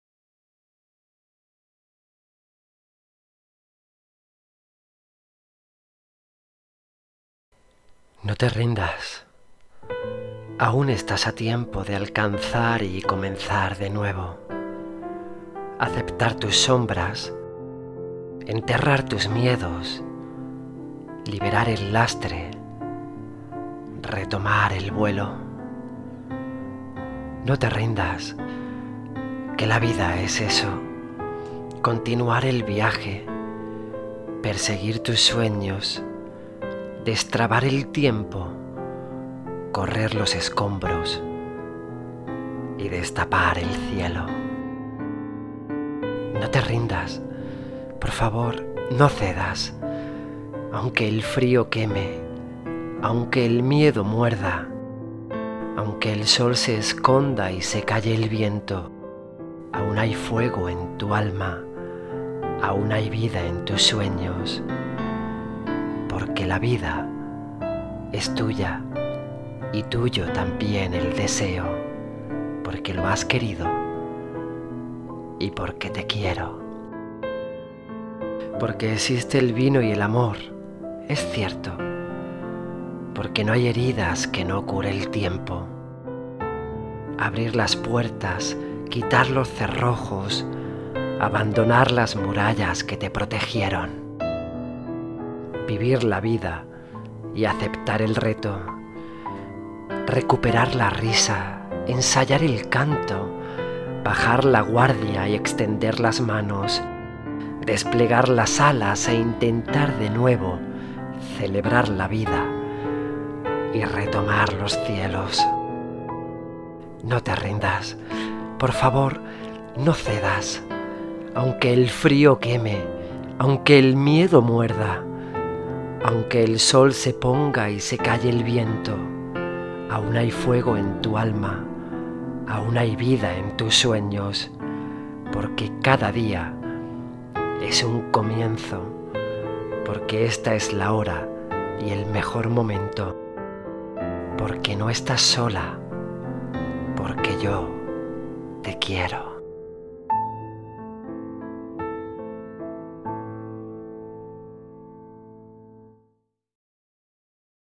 NO TE RINDAS - (de Mario Benedetti) Recitada por José Daniel Rojo y Carlos Castro al piano
Versión del bello texto de Mario Benedetti "No te rindas", acompañado al piano por Carlos Castro.